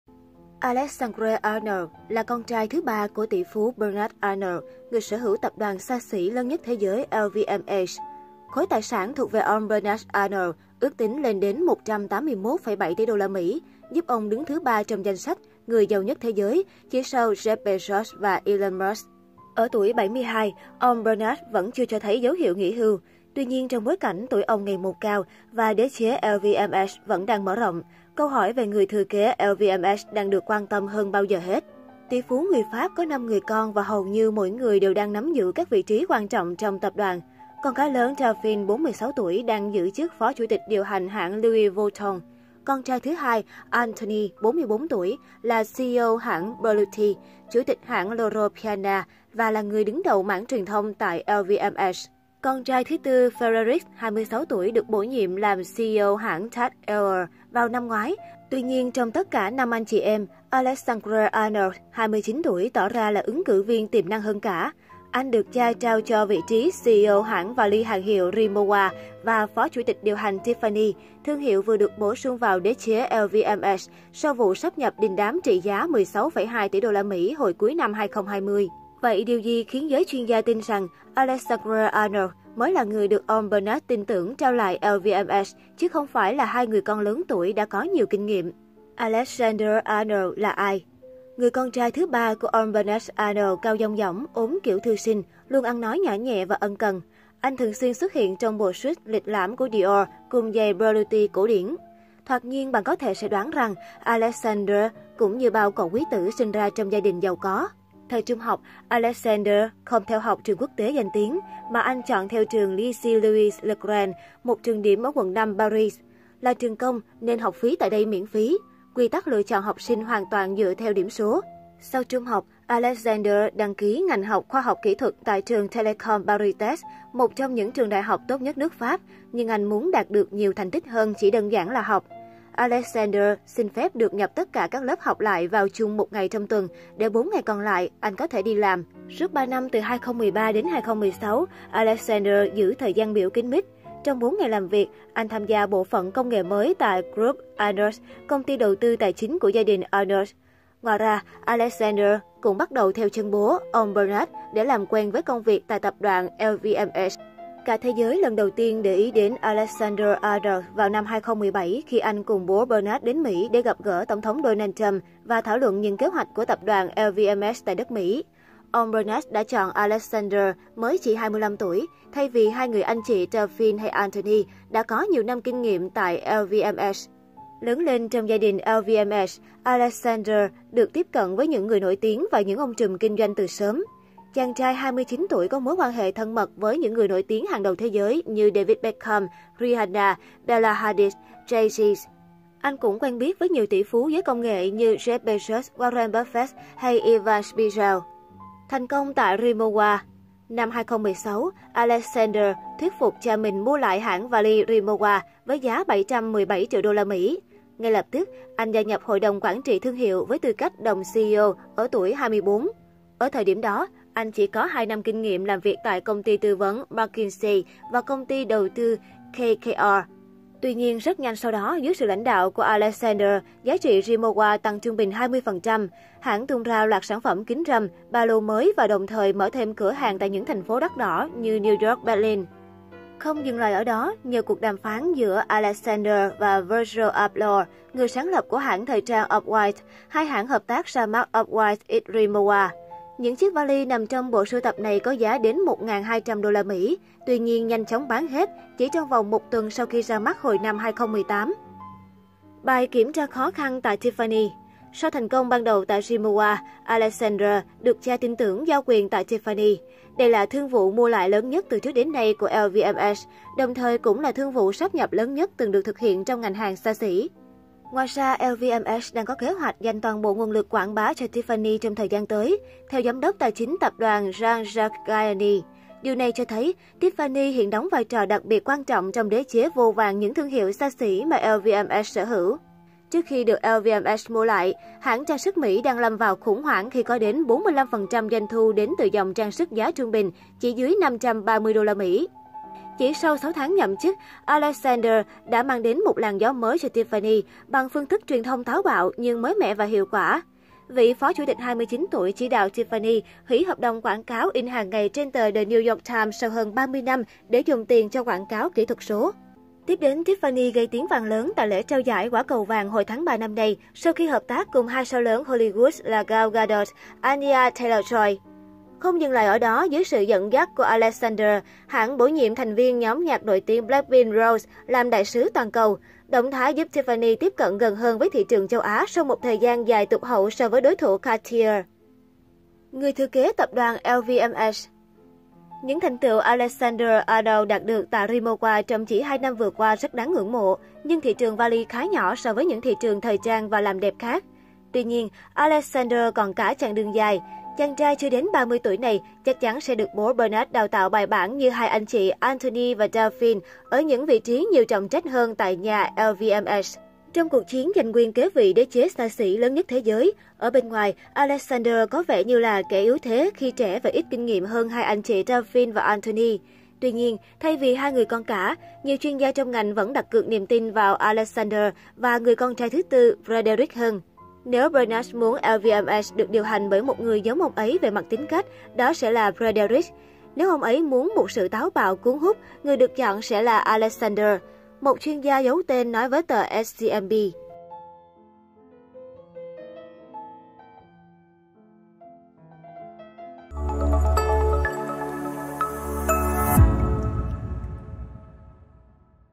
Alexandre Arnault - Công tử trẻ tuổi, người thừa kế tiềm năng của đế chế LVMH
Alexandre Arnault là con trai thứ ba của tỷ phú Bernard Arnault - người sở hữu tập đoàn xa xỉ lớn nhất thế giới LVMH. Khối tài sản thuộc về ông Bernard Arnault ước tính lên đến 181,7 tỷ USD - giúp ông đứng thứ 3 trong danh sách người giàu nhất thế giới, chỉ sau Jeff Bezos và Elon Musk.